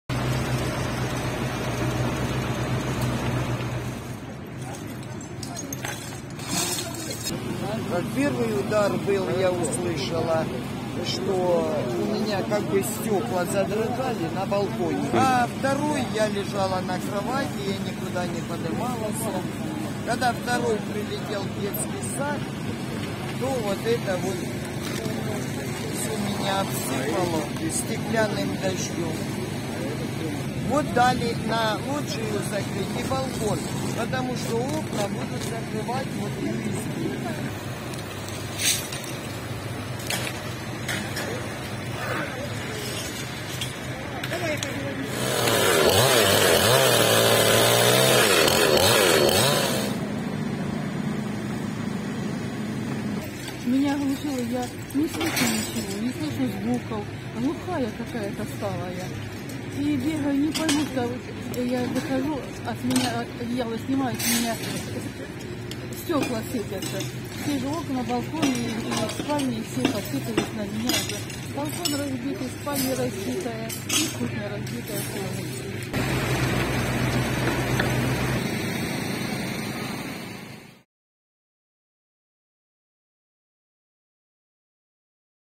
5 людей поранено внаслідок нічного удару по Дружківці
Росіяни знову поцілили у житловий квартал міста. Першою ракетою пошкоджено 4 багатоповерхівки – в одній із них обвалилися сходові прольоти 1 і 2 поверхів в одному під’їзді. Друга ракета влучила у дитсадок.

Всім постраждалим надана вся необхідна медична допомога. Власники пошкоджених квартир забезпечені плівкою та плитами для перекриття вікон.

Попри те, що Дружківка – місто, відносно віддалене від лінії фронту, вона все одно потерпає від російських окупантів. На Донеччині немає абсолютно безпечних місць!

Евакуюйтесь! Збережіть своє життя!